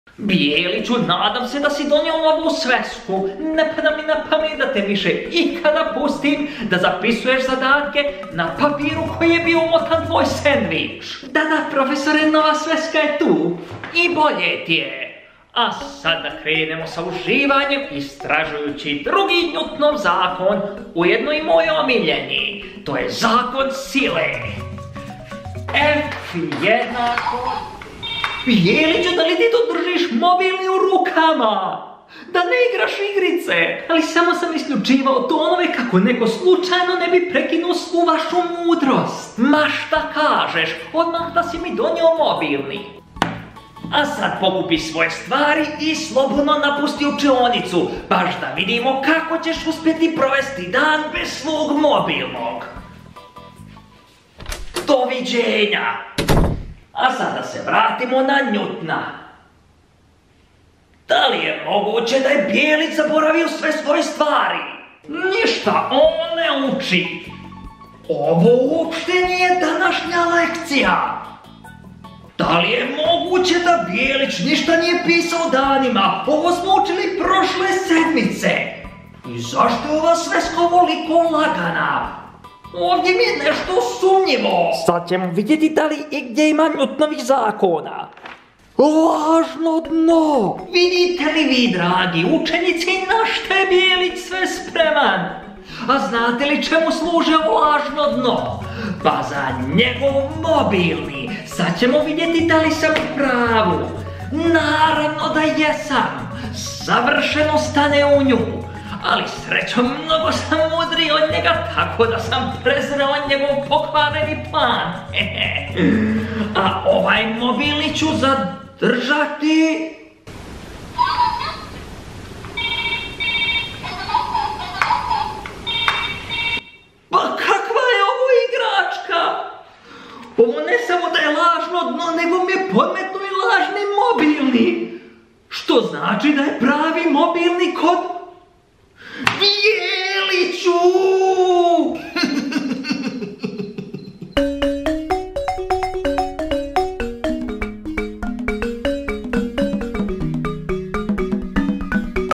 PROFESOR MI JE ODUZEO MOBILNI
Danas mi je profesor oduzeo mobilni, međutim izveo sam jedan zanimljivi trik, pogledajte video i otkrijte koji!